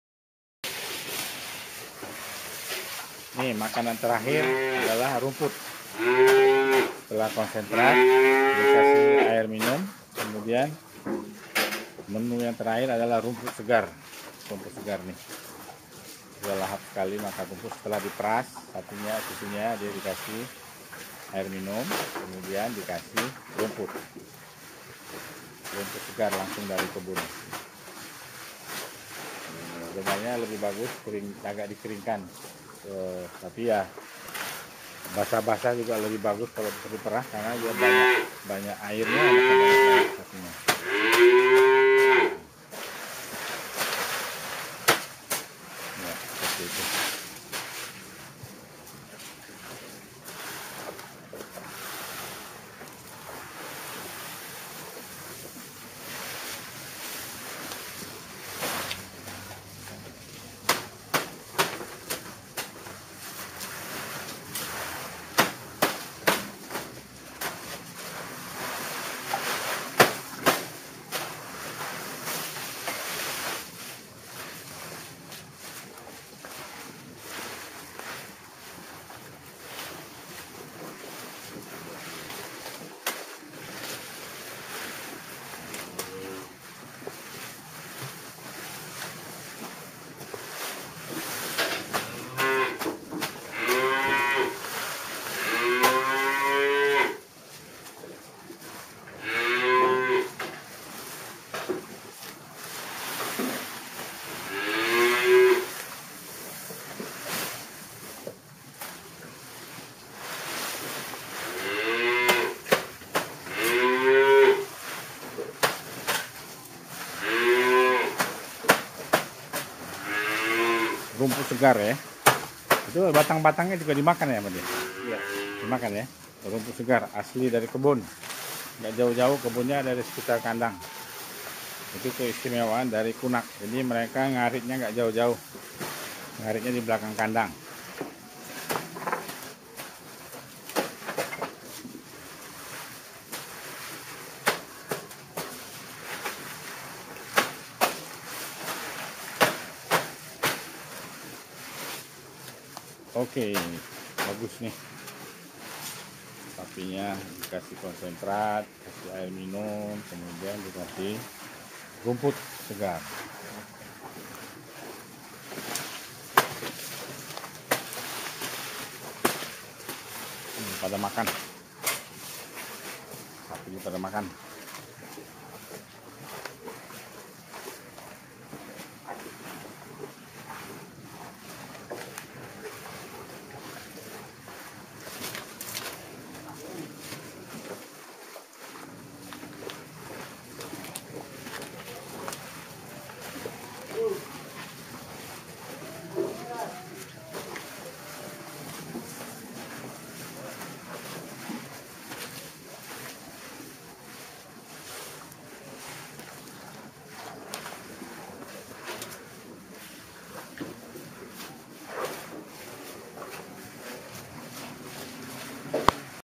Kandang Sapi Perah Pak Bubun Kunak Bogor #05 (setelah konsentrat, rumput tetap diberikan)
Peternakan sapi perah secara nasional mengalami penurunan jumlah populasi karena banyak peternak mengalami kerugian akibat jumlah liter susu yang dihasilkan per hari per ekor sapi rendah.
Menurut Pak Bubun kalau seekor sapi perah menghasilkan hanya 10 liter atau dibawah 10 liter maka dipastikan peternak akan rugi.
Keadaan ini diperparah oleh rendahnya harga jual susu segar ke Koperasi Susu atau pihak ketiga lain.
Beberapa point penyebab rendahnya susu per hari adalah:
- kekurangan rumput (rumput yang diberikan kurang)
- harga konsentrat sebagai makanan tembahan mahal
- air minum yang diberikan kurang
- penyakit yan berakibat sapi tidak nyaman dalam kandang

Beberapa kiat peternak untuk bertahan hidup adalah menciptakan harga jual susu tinggi sebagai berikut:
- mengolah susu segar menjadi yoghurt
- mengolah susu segar menjadi keju
- pasteurisasi susu segar dan menjualnya secara eceran
- mencari langganan dari rumah tangga.

Ada beberapa video saya yang mencoba menggambarkan peternakan sapi perah di sekitar Bogor, permasalahan, manajemen pemeliharaan sapi, serta kiat bertahan hidup.
Selamat mengikuti semoga ada manfaatnya dan terhibur bagi para masyarakat umum yang bukan peternak sapi perah.

Terima kasih
Rajali Yahya
Dokter Hewan Jalanan
Vet On The Road